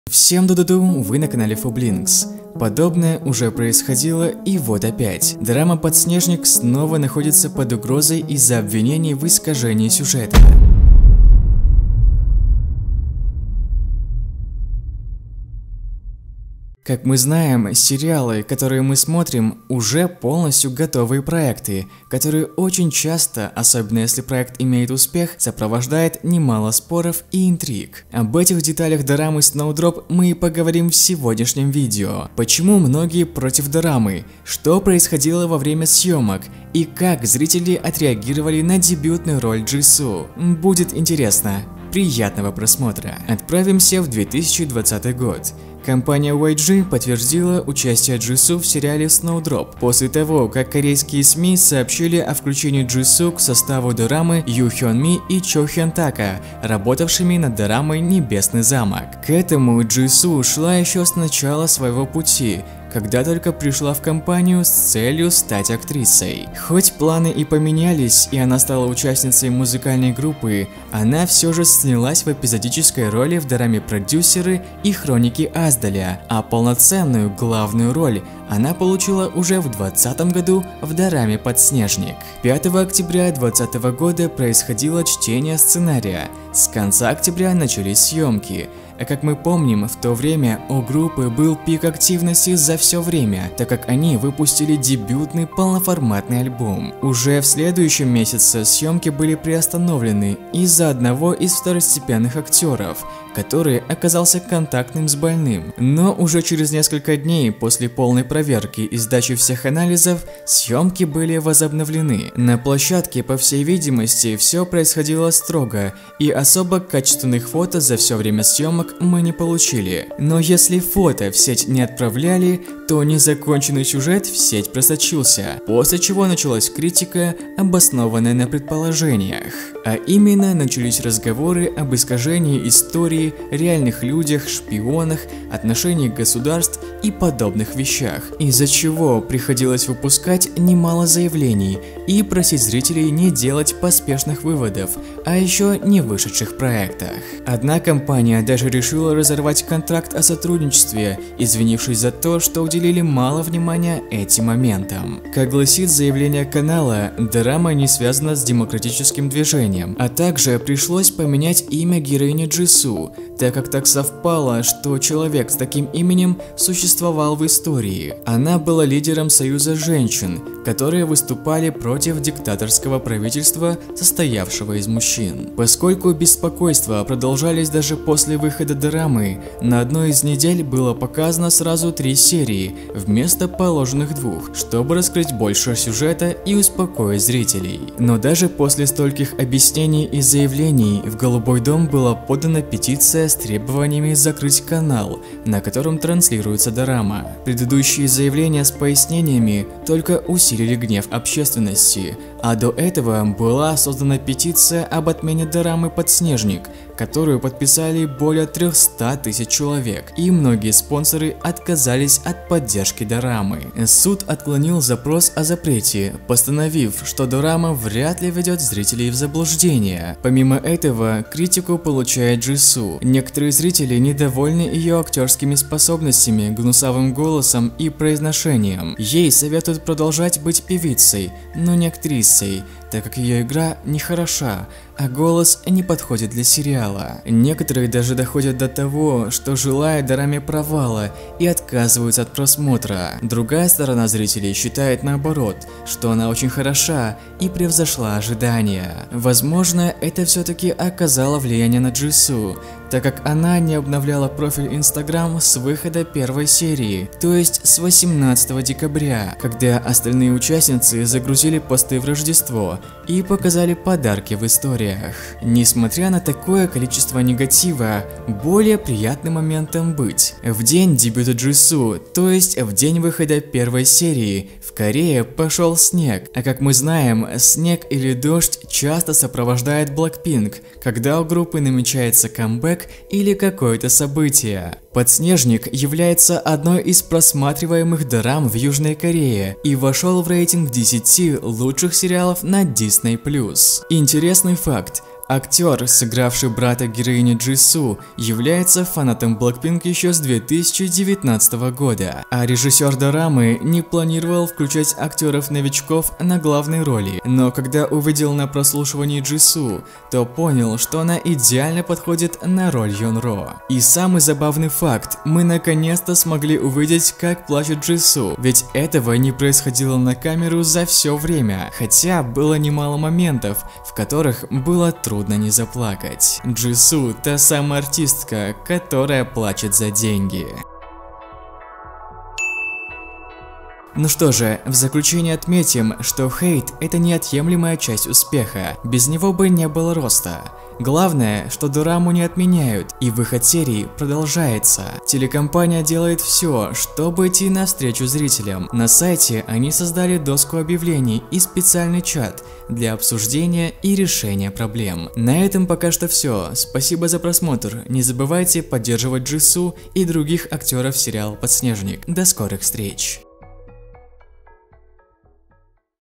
SNOWDROP: Петиции за отмену, Критика навыков Джису и Интриги вокруг дорамы
ДоБро поЖаловать! Дорама подснежник с Джису в главной роли, говорим об отзывах зрителей, причинах ненависти, навыках Джису и многом другом.